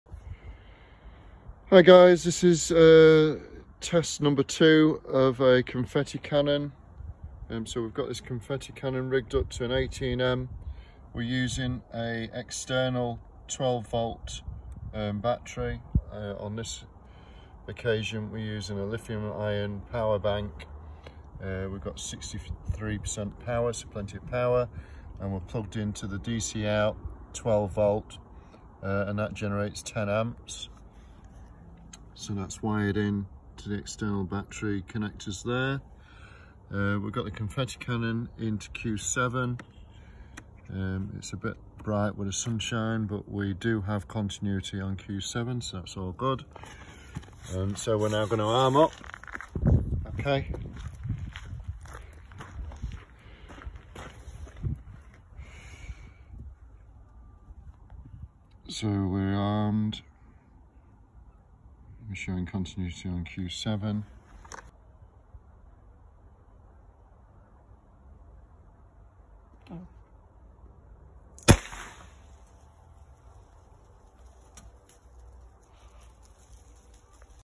Confetti Cannon testing good for gender reveals! Rainbow Fireworks
We have run various trials to get confetti cannons to fire using the cobra firing system. This is our second test using external power to deliver 12v 10amp. Fired perfectly apart from a slight operator error - trying to fire from cue 1 instead of cue 7!!